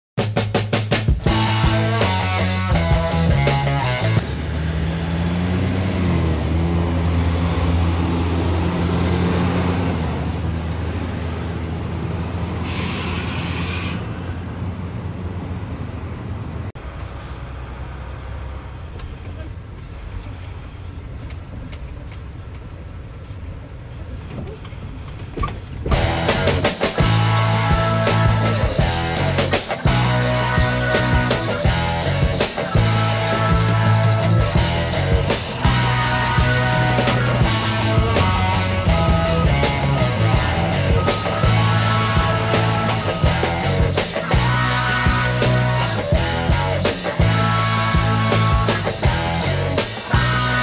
Cool Truck Loads 90 Grader + Paving Equipement
R. W. Tomlinson Limited hauling a Grader and Paving Equipment.